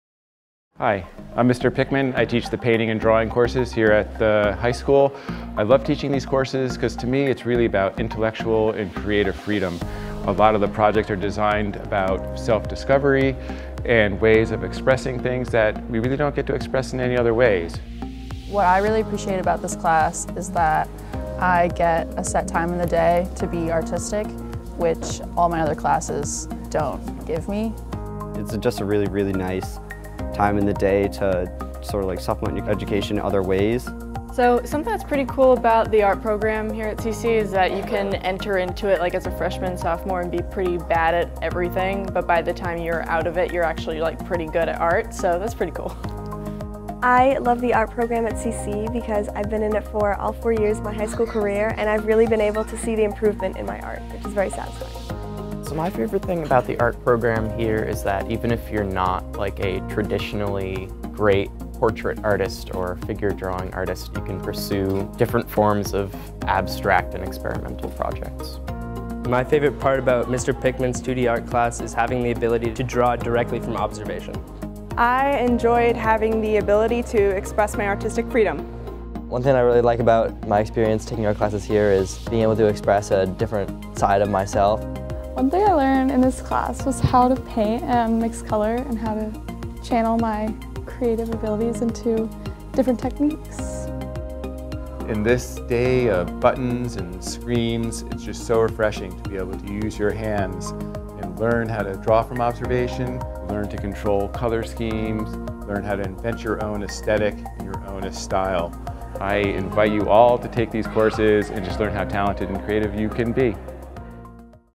Why Art Matters
Joe Pickman and his students talk about why art matters to them.